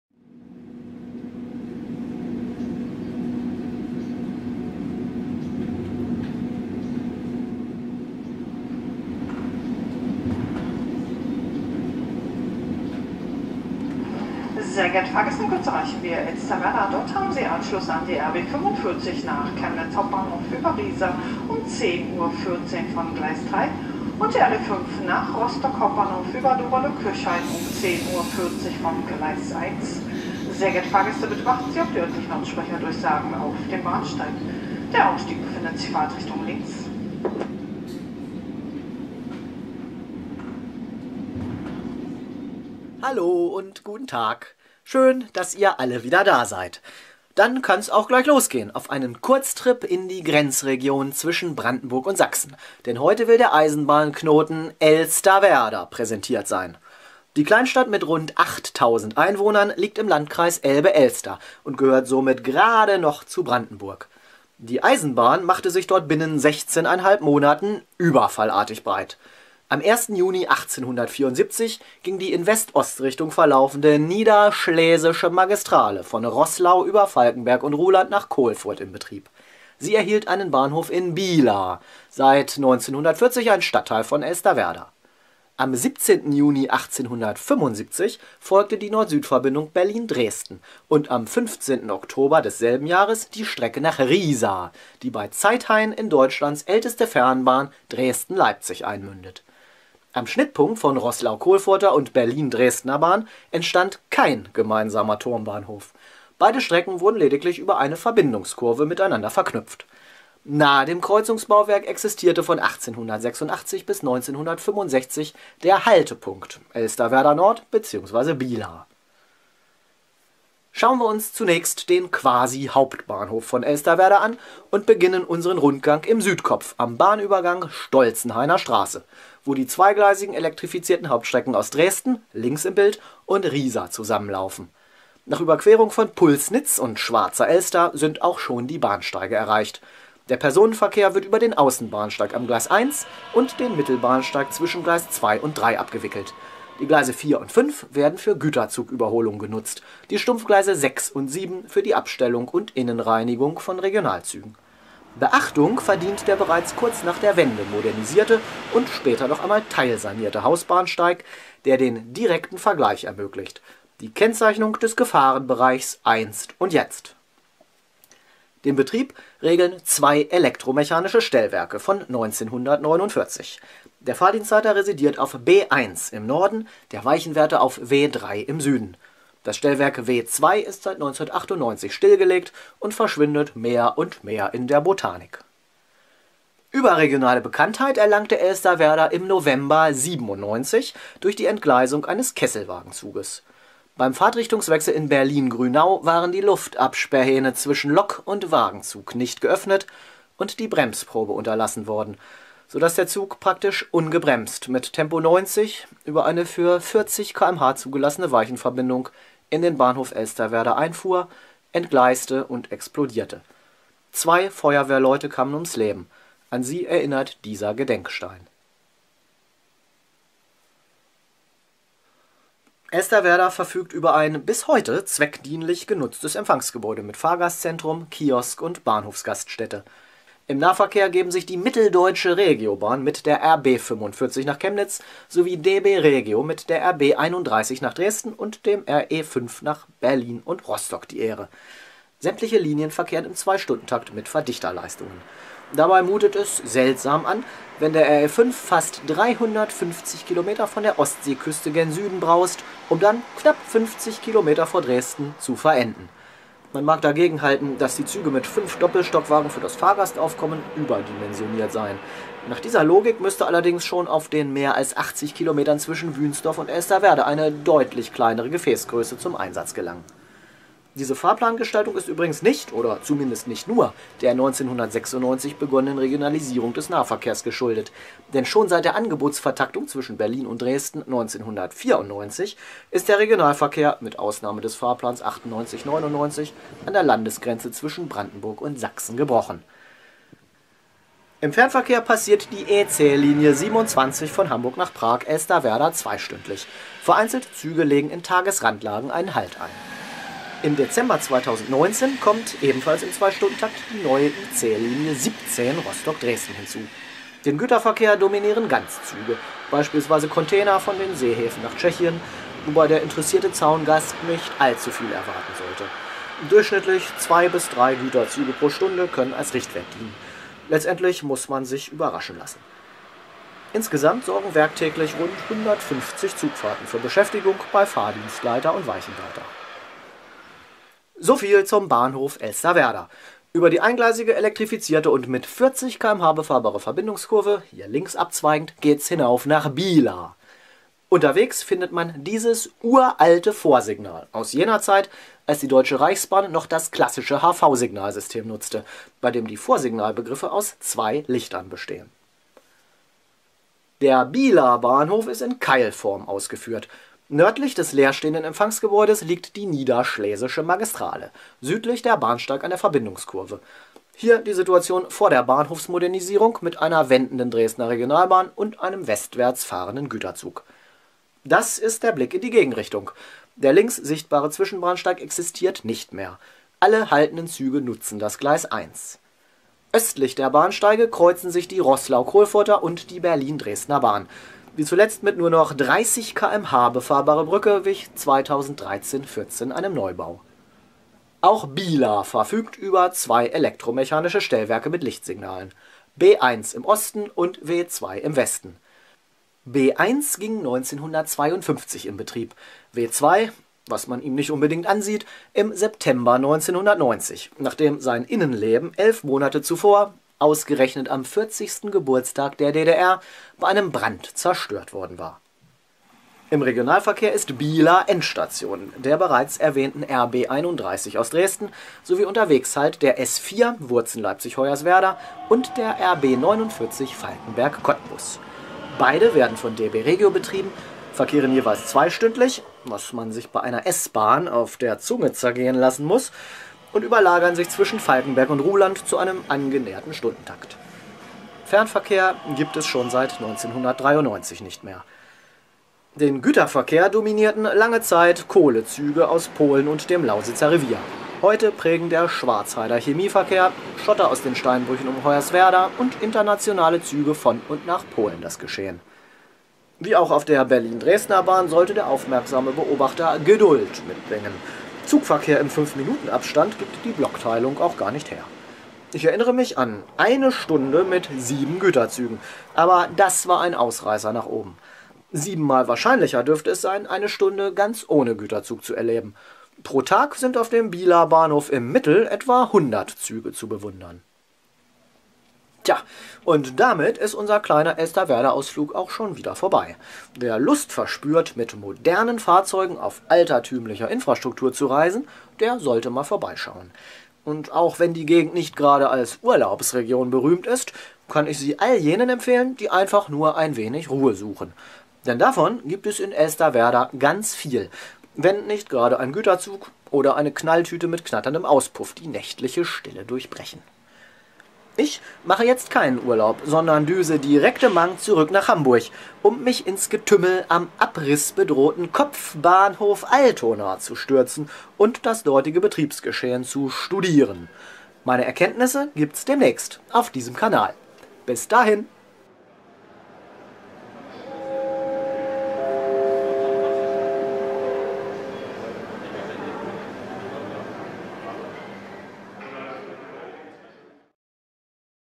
Kleinstadt-Knoten: Elsterwerda und seine Bahnhöfe
Nahe der brandenburg-sächsischen Landesgrenze liegt der kleine Eisenbahnknoten Elsterwerda. Hier kreuzen sich die über eine Verbindungskurve verknüpften Hauptbahnen Berlin–Dresden und Roßlau–Kohlfurt; daneben existiert noch die Strecke nach Riesa.

Dieser Film beschreibt die Geschichte der Eisenbahn in Elsterwerda, präsentiert die Infrastruktur und beleuchtet das aktuelle Betriebsgeschehen.